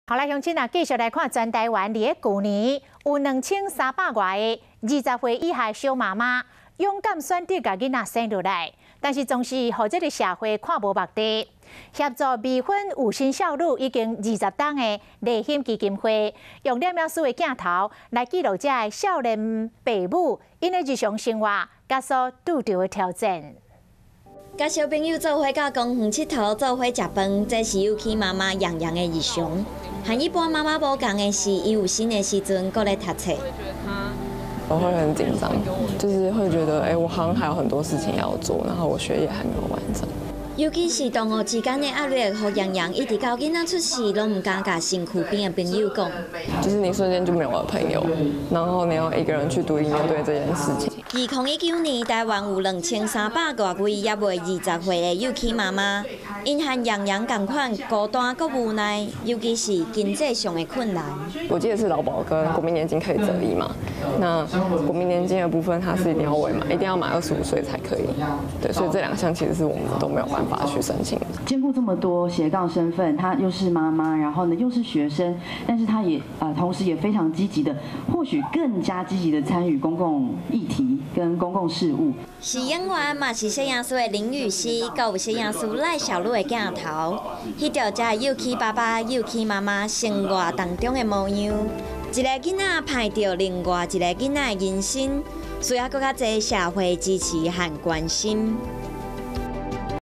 【台語新聞】攝影師記錄生活 小媽媽日常曝光 | 華視新聞 20201005
去年全台有2300多名20歲以下的小媽媽，勇敢選擇生下孩子，卻總是被社會用異樣眼光看待。協助未婚懷孕少女已20年的勵馨基金會，用攝影師的鏡頭記錄下小爸媽真實生活樣貌，以及他們在日常中遇見的各種挑戰。